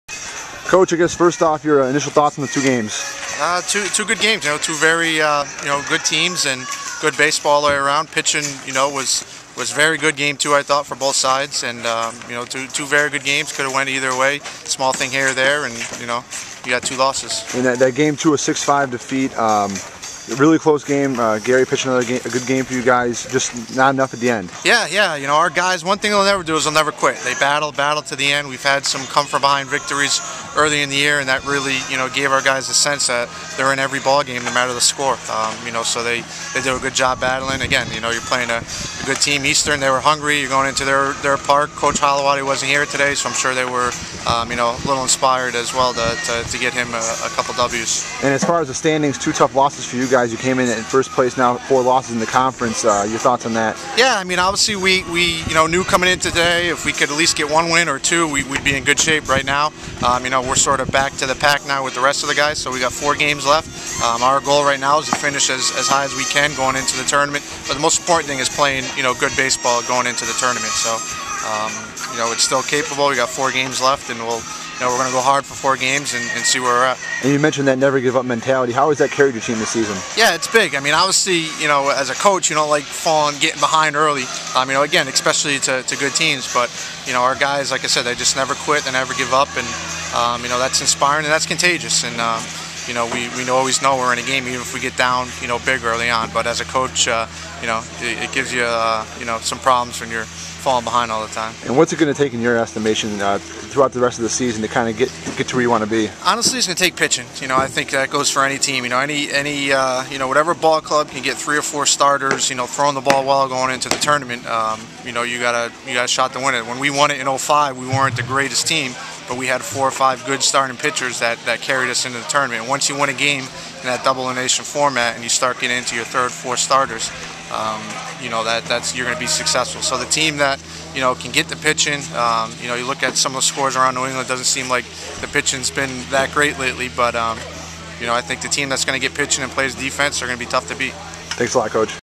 RIC Head Coach Scott Virgulak
Scott Virgulak shares his thoughts on the doubleheader with ECSU and his outlook on the remainder of the season.